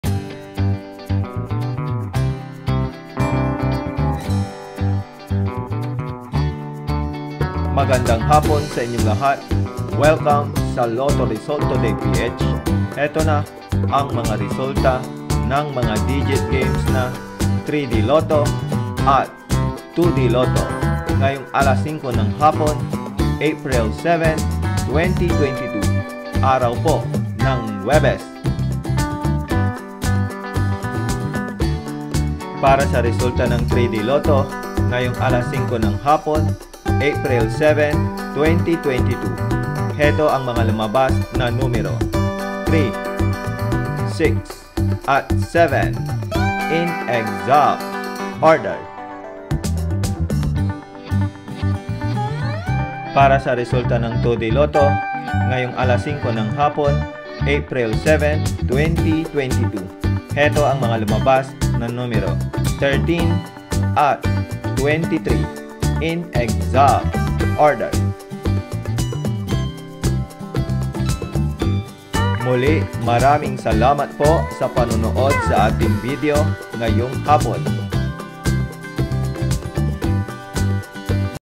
Lotto Result Today 5pm Draw April 7 2022 Swertres Ez2
Here are the PCSO Lotto Result Today 5pm Draw April 7, 2022

The Lotto Games this 5pm Afternoon are:
3D Lotto (Swertres)
2D Lotto (Ez2)